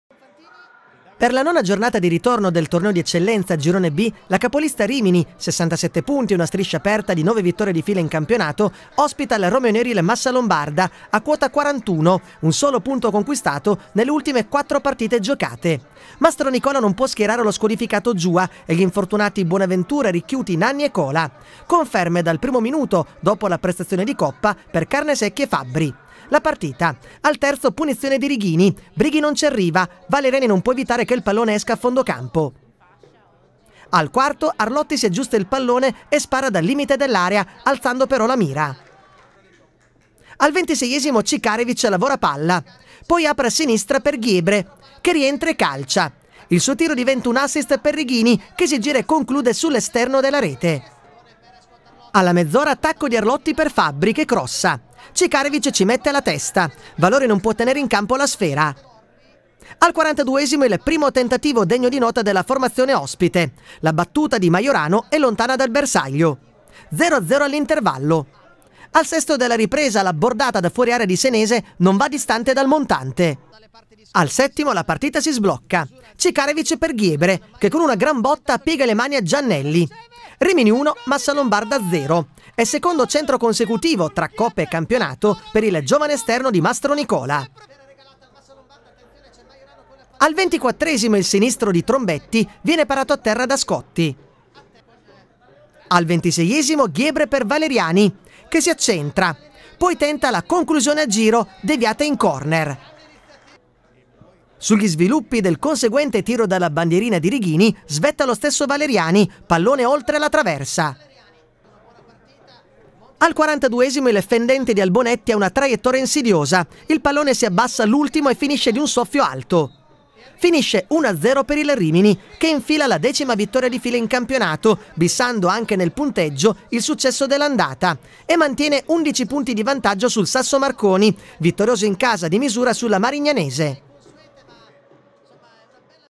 Icaro Sport. Rimini-Massa Lombarda 1-0, il servizio
Il servizio sulla partita Rimini-Massa Lombarda (1-0, con gol partita di Guiebre al 7' st) a cura di Roberto Bonfantini.